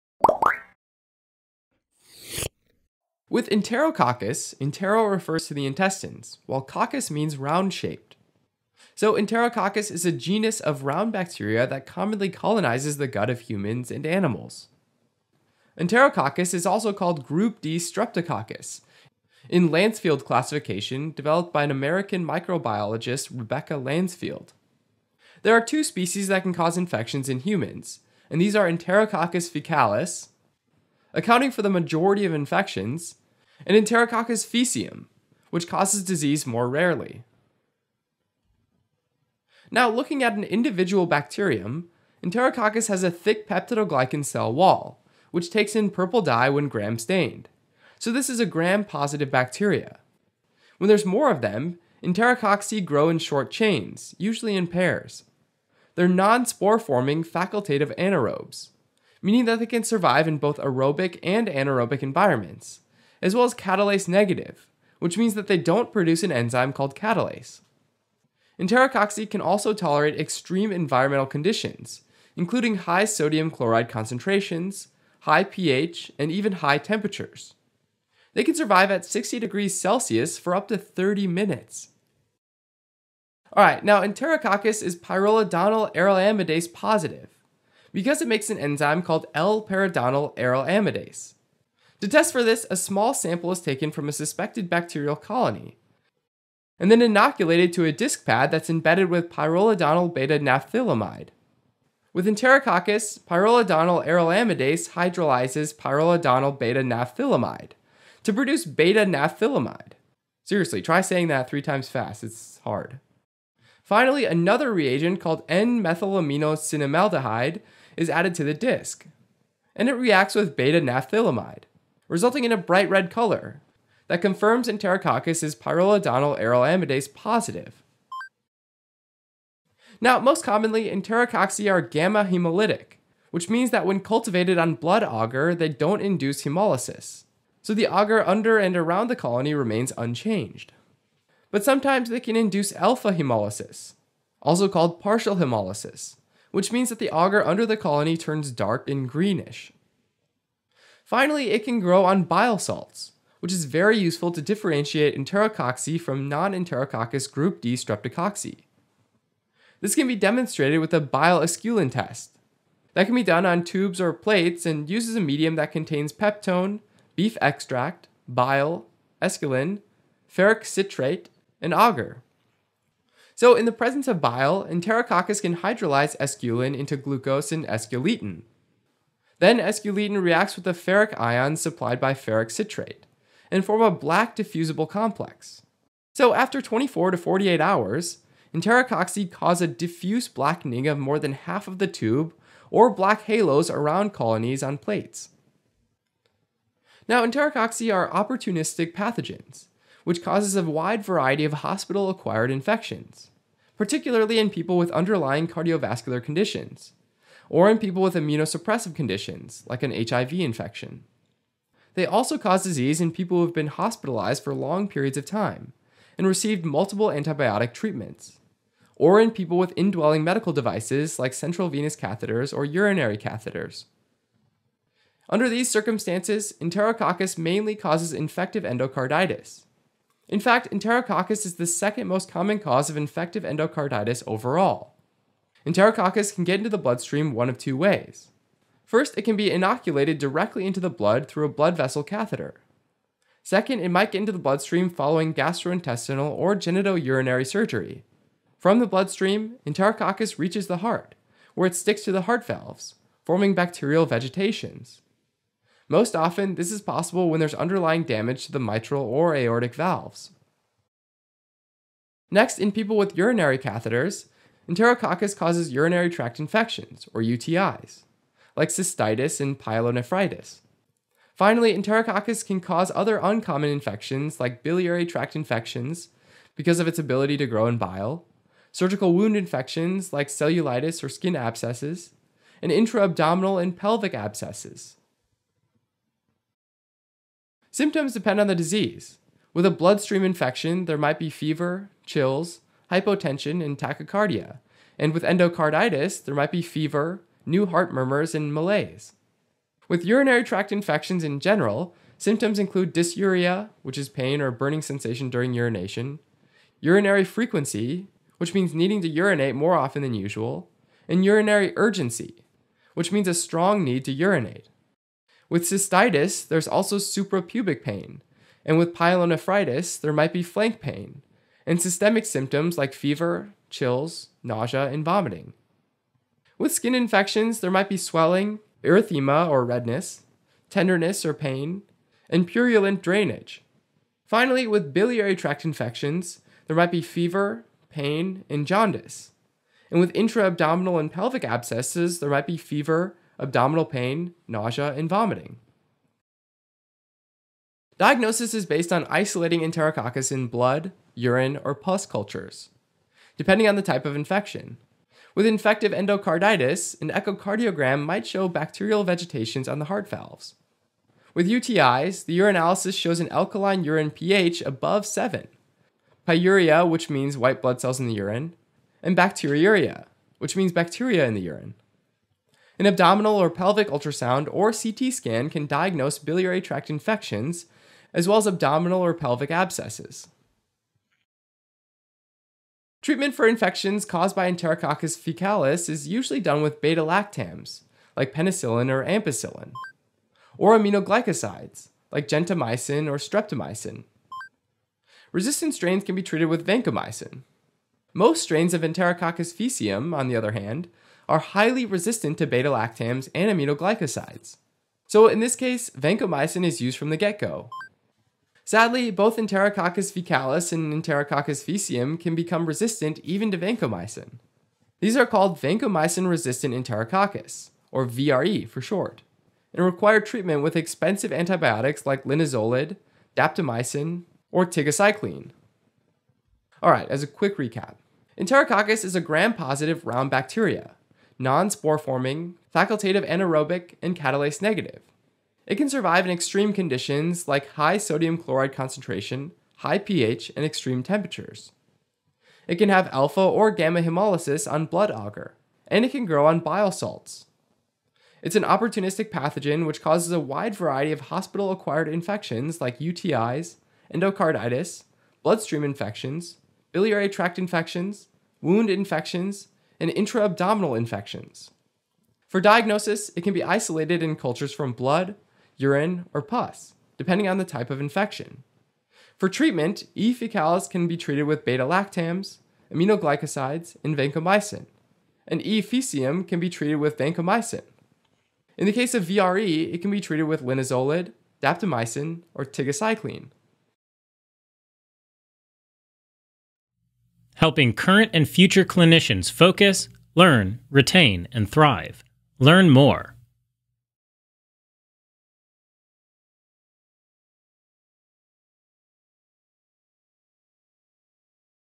Enterococcus- causes, symptoms, diagnosis, treatment, pathology
What is enterococcus? With Enterococcus, entero- refers to the intestines, while -coccus means round shape. So Enterococcus is a genus of round bacteria that commonly colonizes the gut of humans and animals.